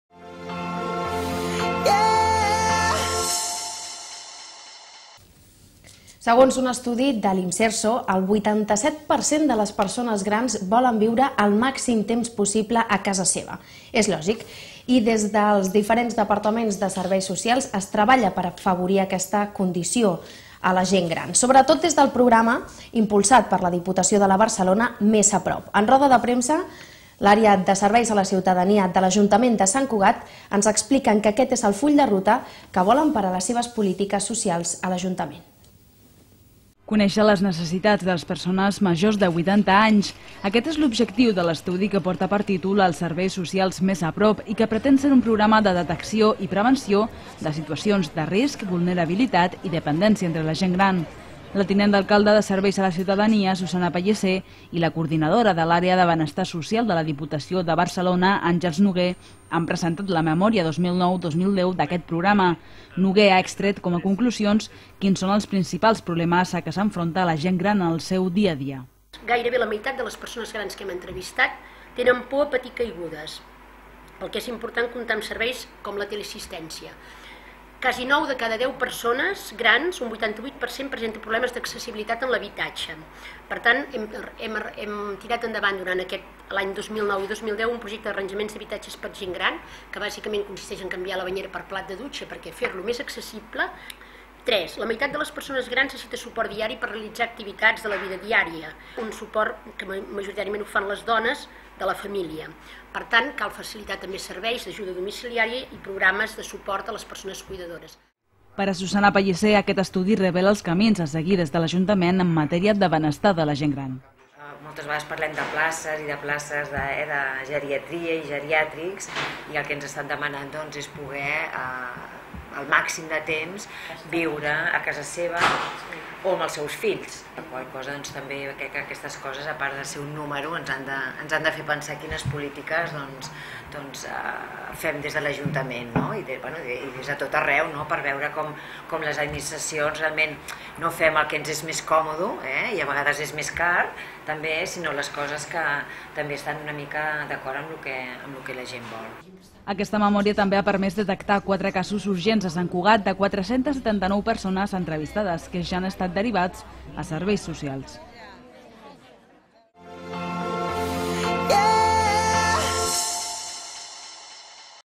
L'estudi "Els serveis socials més a prop" revela les necessitats de la gent gran a Sant Cugat.
Segons un estudi de l'IMSERSO, el 87% de les persones grans volen viure a casa seva tant de temps com sigui possible. Aquesta és una voluntat que els serveis socials pretenen afavorir, en especial, el programa "Més a prop" que ha dut a terme la Diputació de Barcelona i que des de la regidoria de Serveis a la ciutadania valoren com un full de ruta a seguir en les polítiques dels ajuntaments en l'atenció a la gent gran.